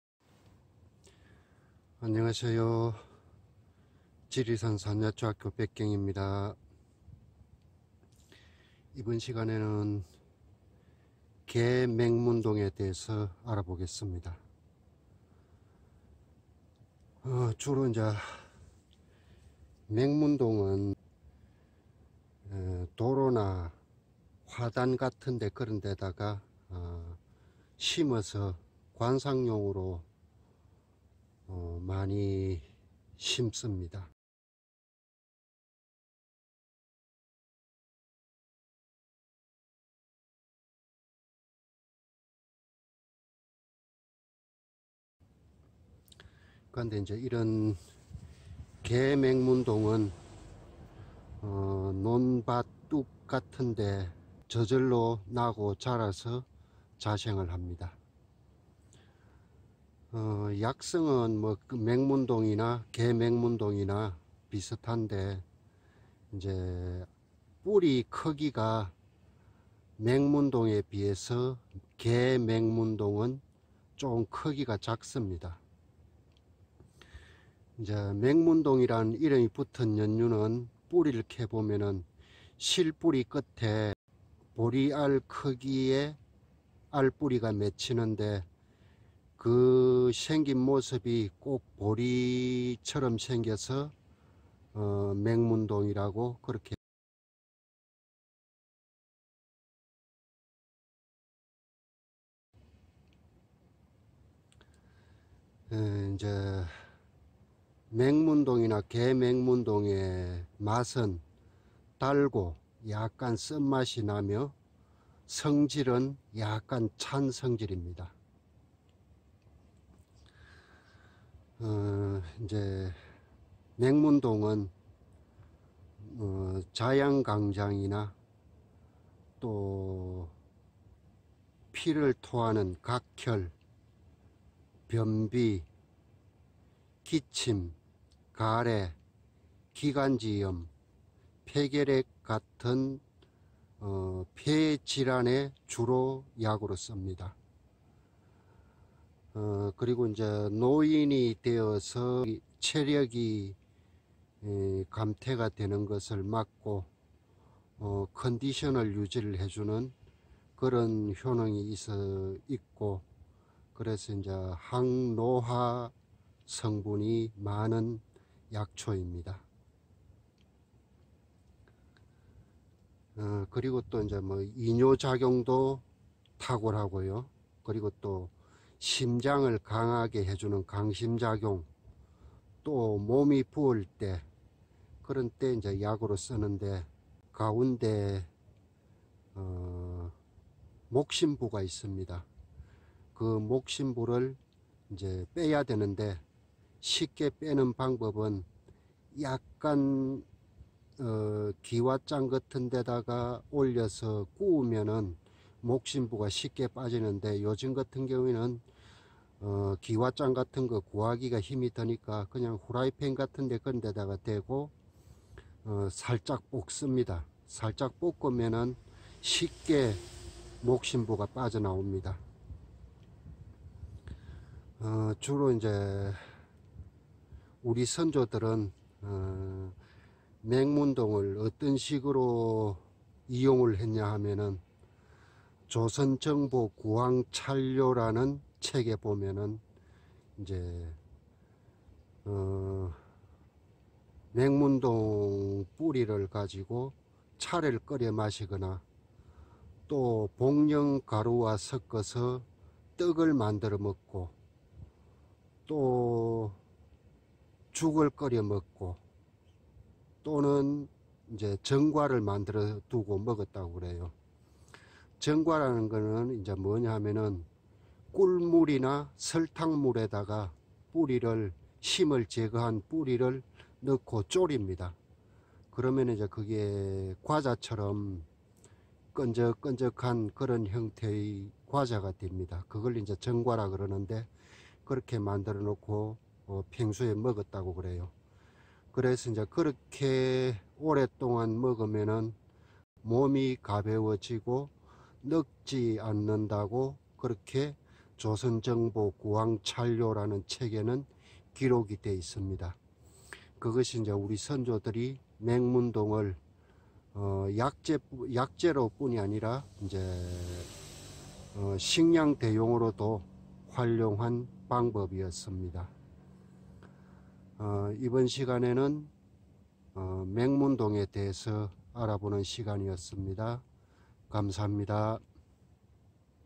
자양강장, 각혈, 기침, 기관지염, 체력감퇴, 강심에 약용하고 식량대용으로 사용하는 개맥문동 맥문동
자양강장, 각혈, 기침, 기관지염, 체력감퇴, 강심에 약용하고 식량대용으로 활용하는 개맥문동